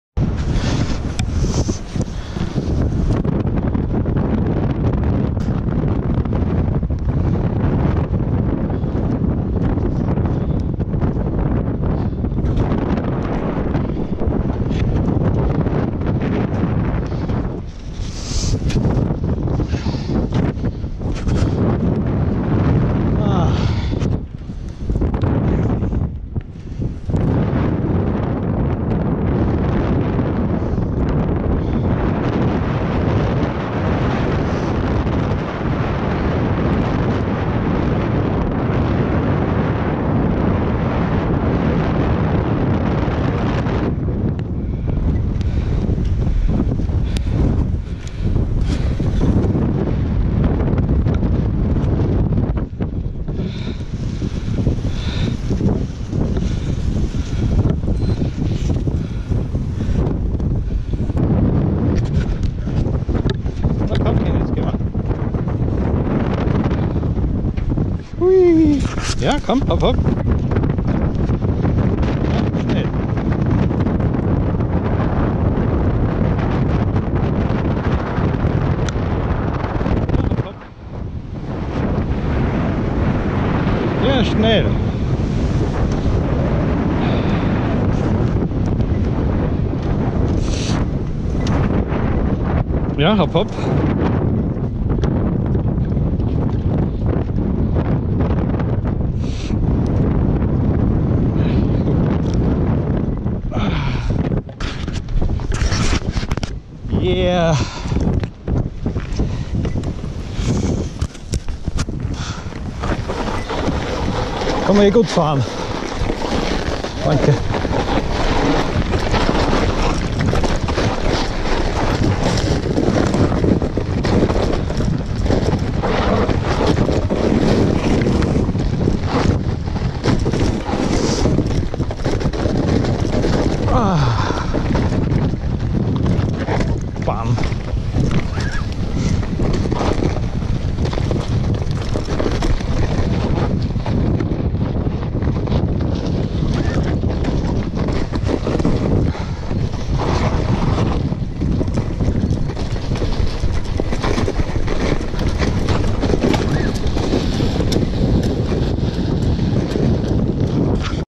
Jubiläumswarte bei Sonnenuntergang + MTB Abfahrt im Schnee
Sturm aus Süd, Minusgrade und ein schöner Sonnenuntergang.
MTB Abfahrt im harten Schnee. Unten dann viel Glatteis.